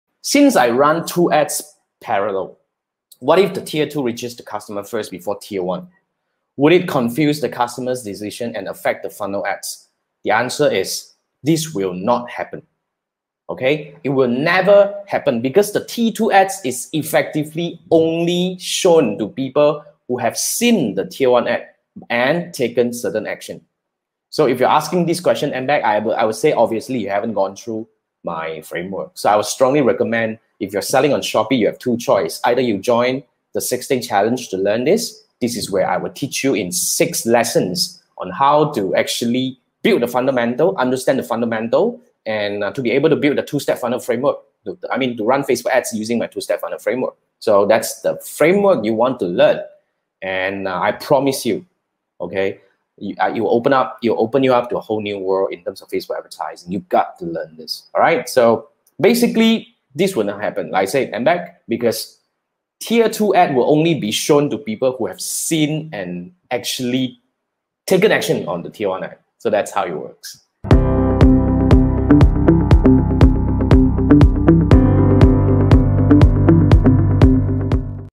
Facebook Retargeting Ads Reach Fresh Audience - Is It Possible? (FB Ads Setting Tutorial)
Is it possible for a retargeting Facebook ads to reach fresh audience first? Here's my take.